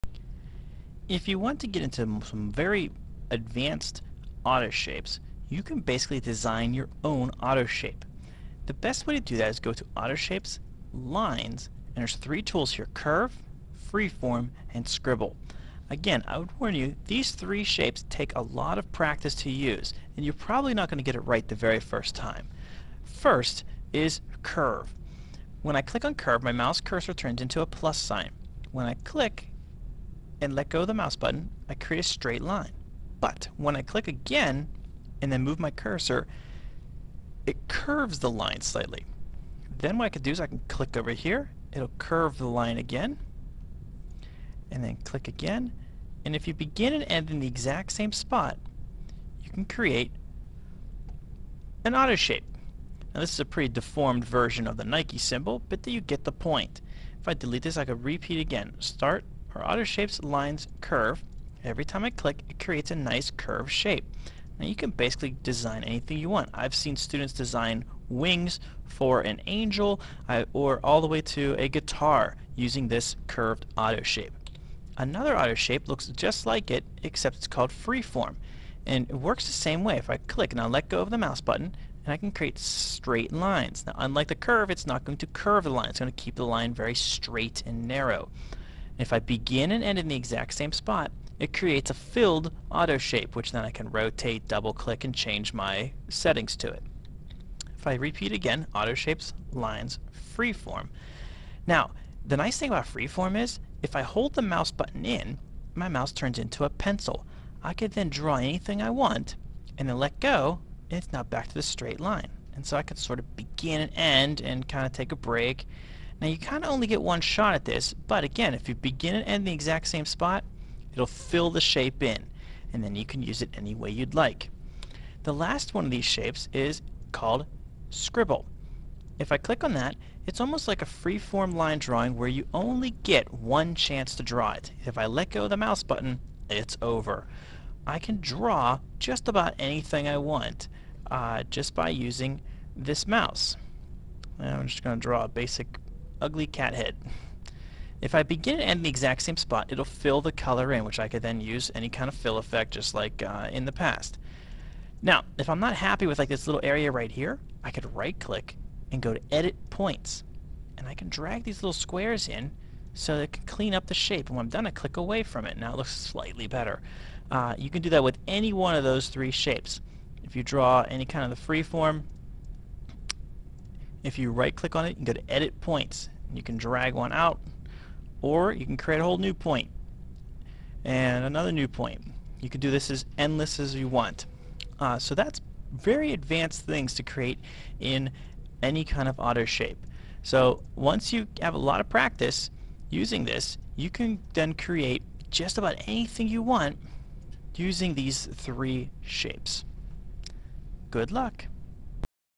Step 1d - Creating Advanced Shapes (Mr. Fogle - Multimedia)
Mr. Fogle, 8th Grade Multimedia Teacher at Seneca Valley Middle School, demonstrates how to create objects with more advanced shapes in PowerPoint.  I use the FreeForm, Curve & Scribble line tools to create these shapes.